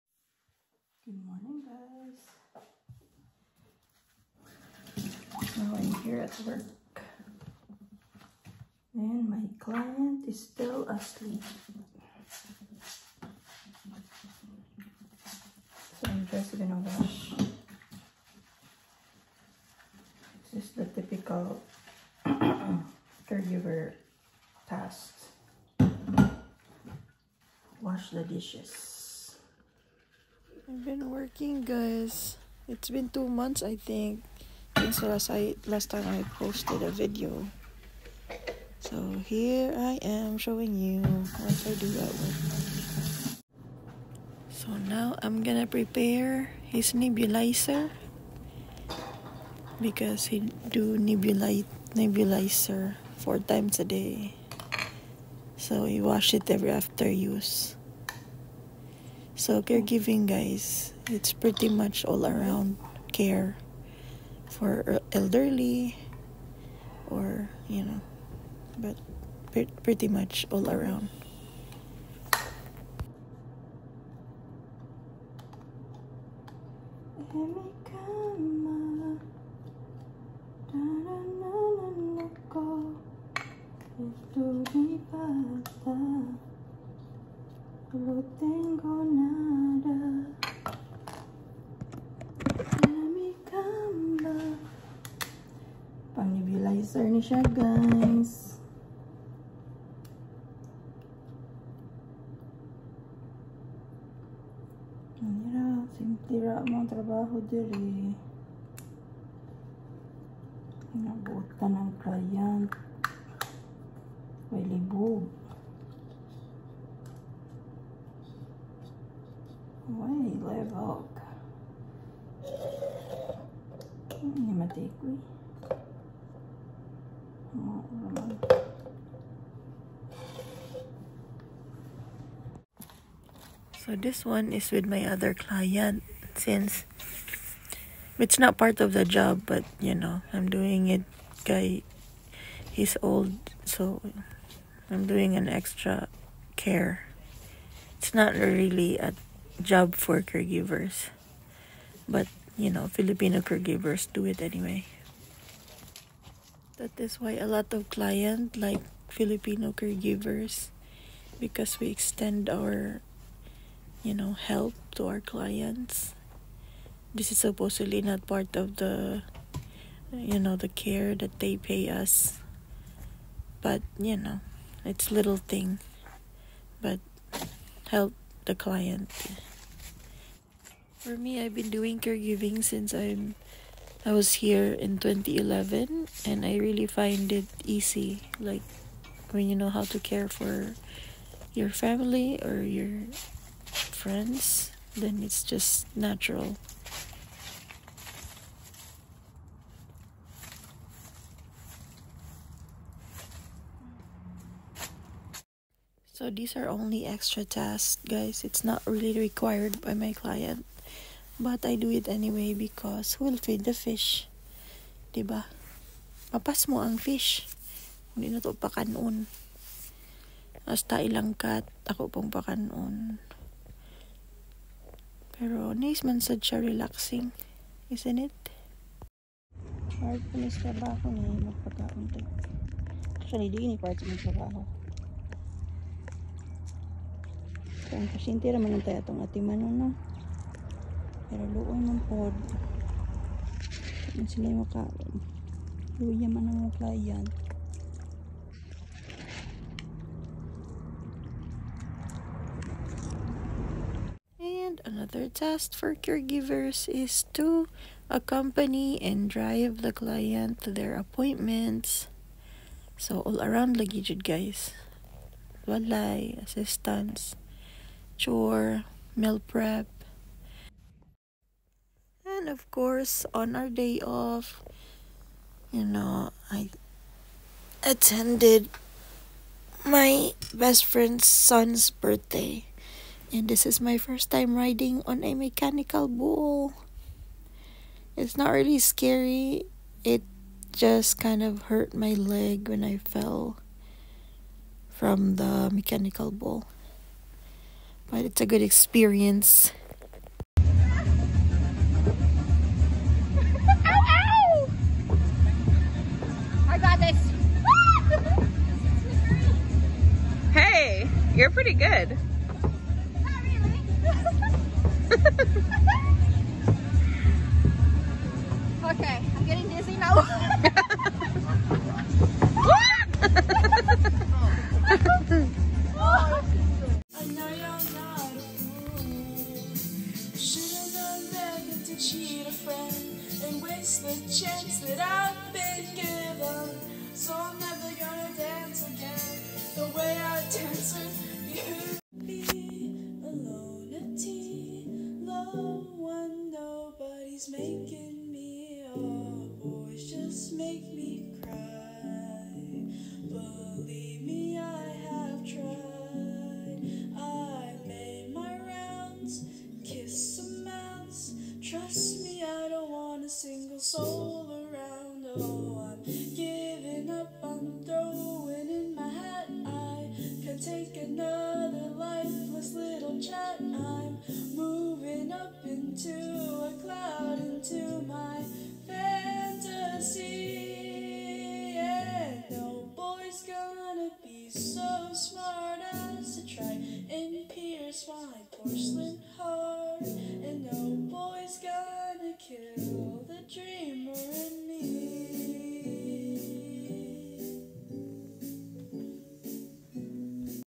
Caregiving in California
Few of my daily  task in caregiving, been a caregiver since 2011 and doing a private care in my clients residence. All around personal assistant.